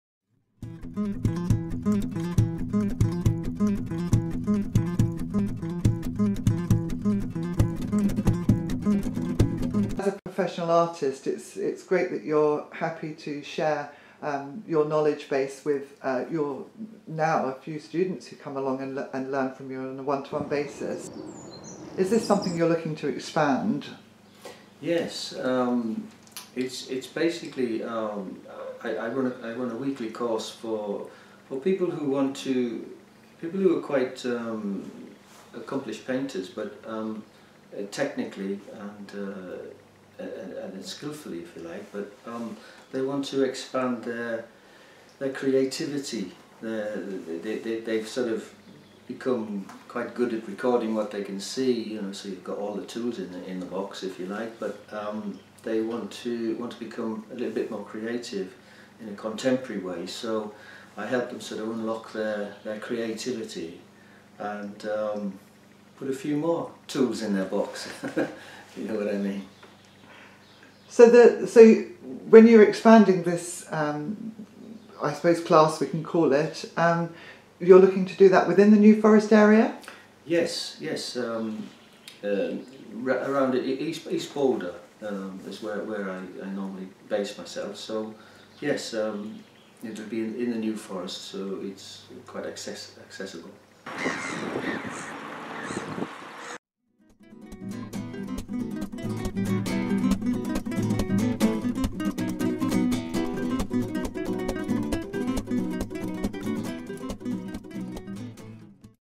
Stephen Powell New Forest Workshops
Contemporary artist Stephen Powell is holding regular workshops in the New Forest. If you want to join in contact him on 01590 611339 and to see Stephen's work,